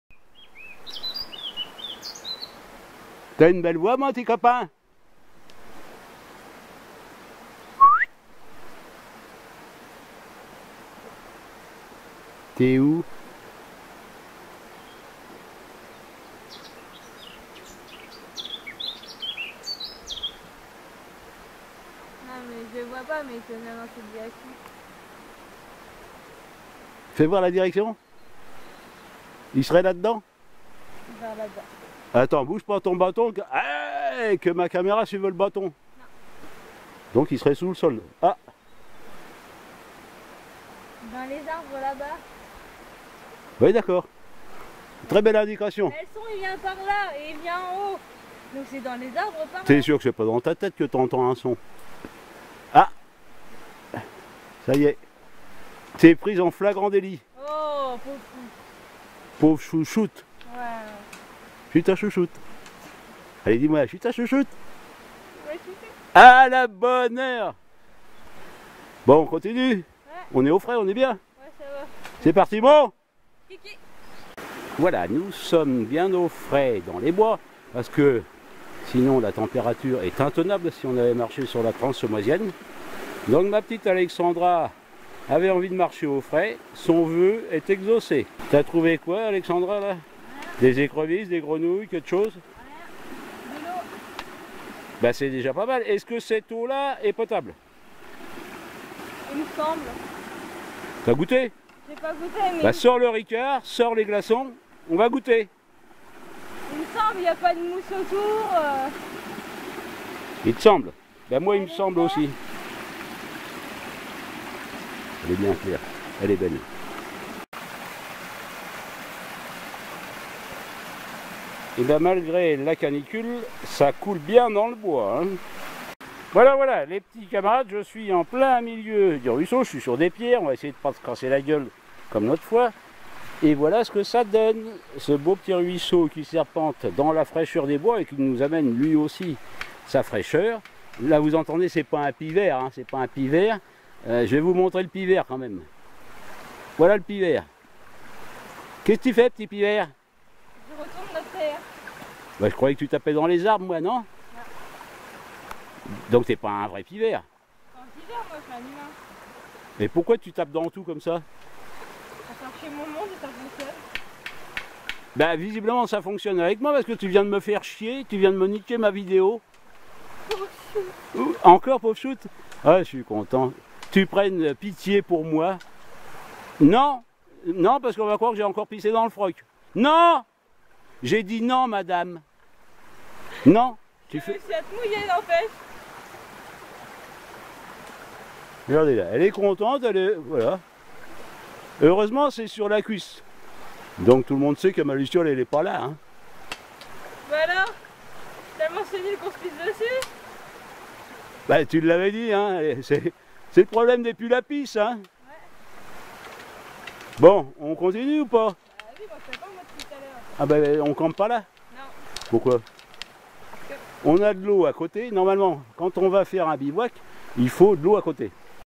Dans la fraîcheur des bois
Une belle rando matinale, dans la fraîcheur des bois, avant la canicule de l'après-midi, avec Ma Petite-Fille Alexandra qui oscille entre humour et taquinerie envers son Papy...!